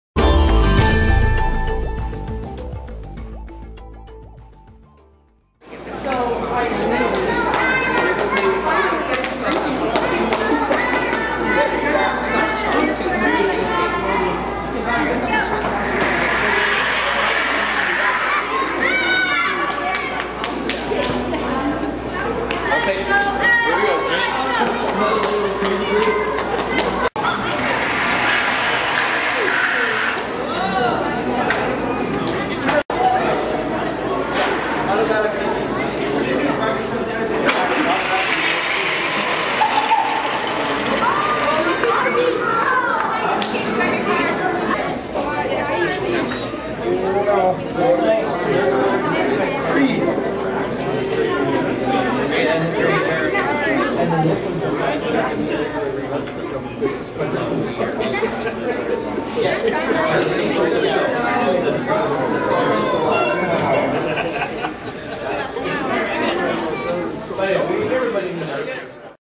Kub Kar Rally
17 Kub Scouts participate in the 2019 Kub Kar Rally at the Swift Current Mall.